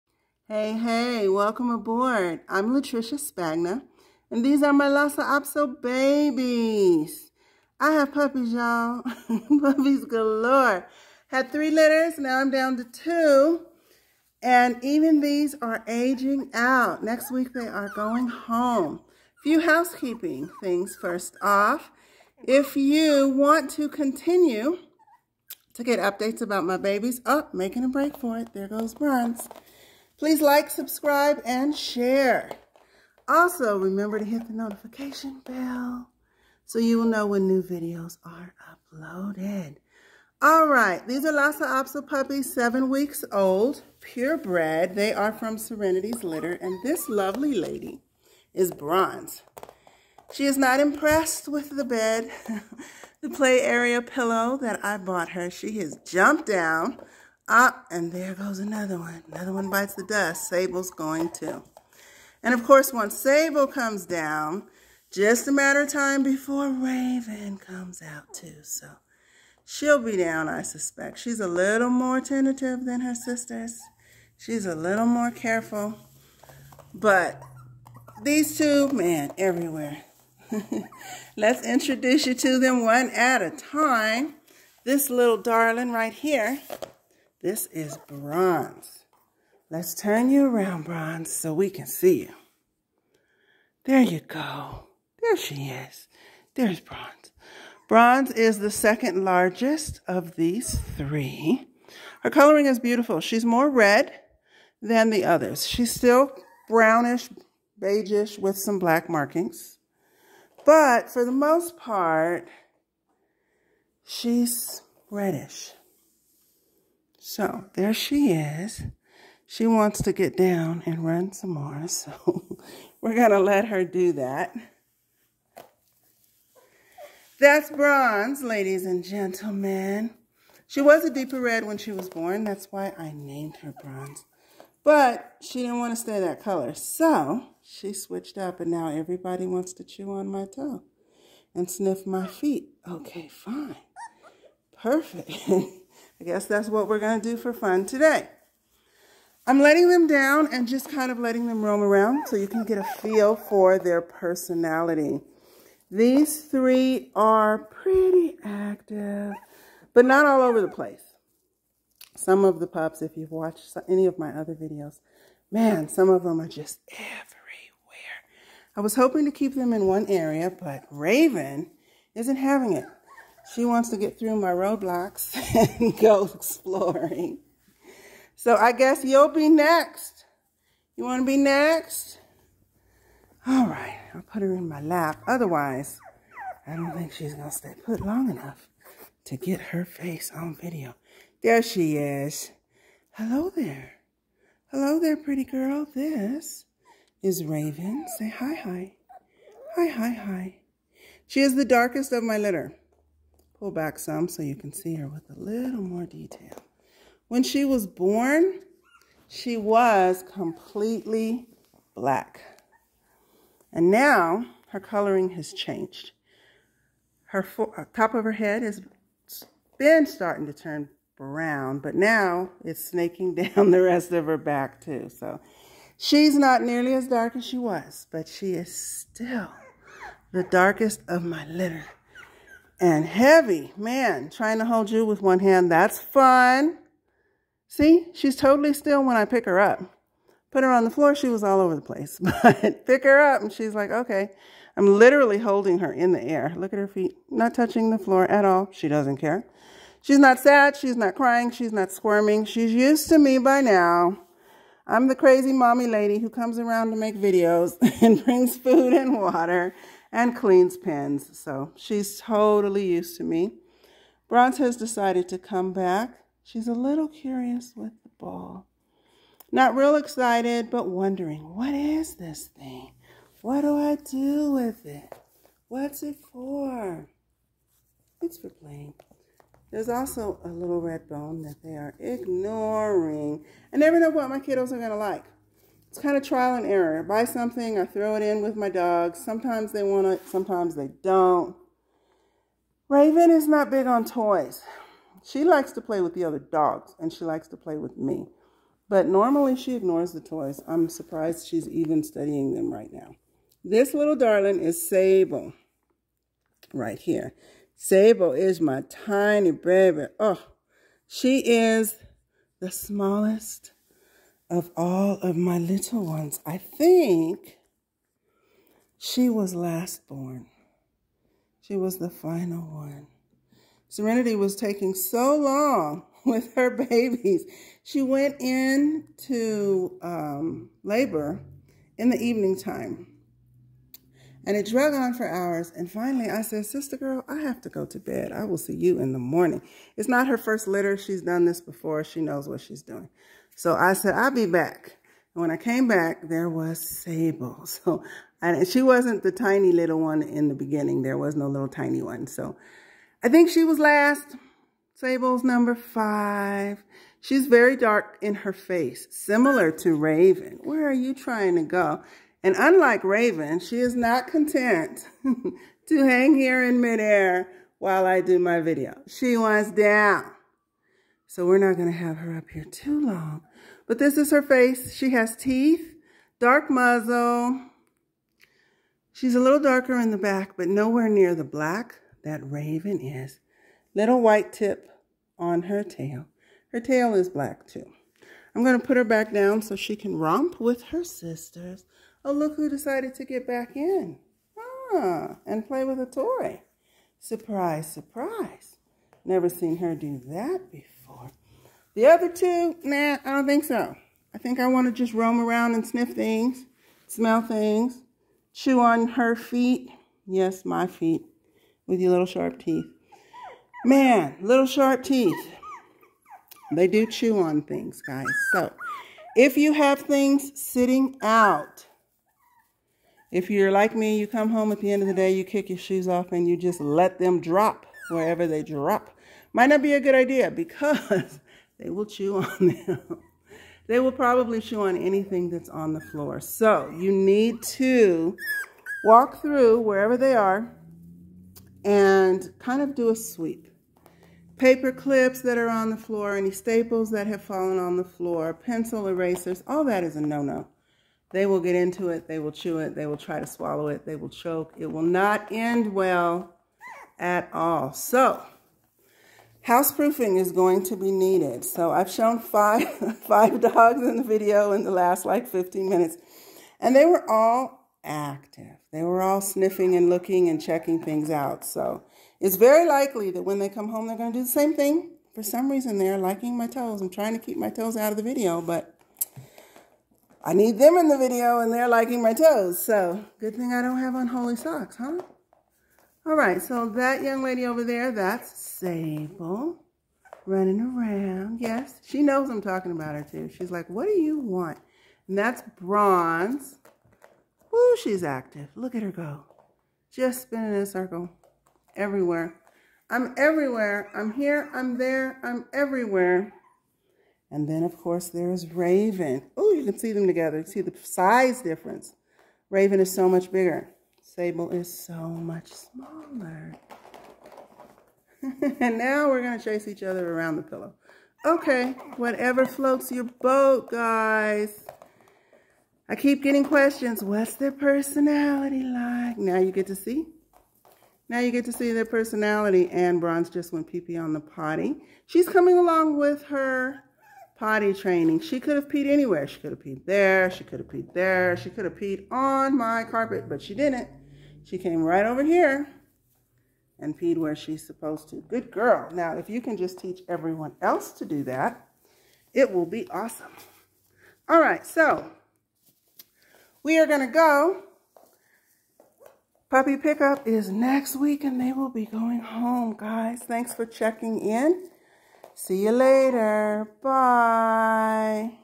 7 week old Lhasa Apso puppies play in the living room (Bronze, Raven, and Sable)
Lhasa Apso puppies playing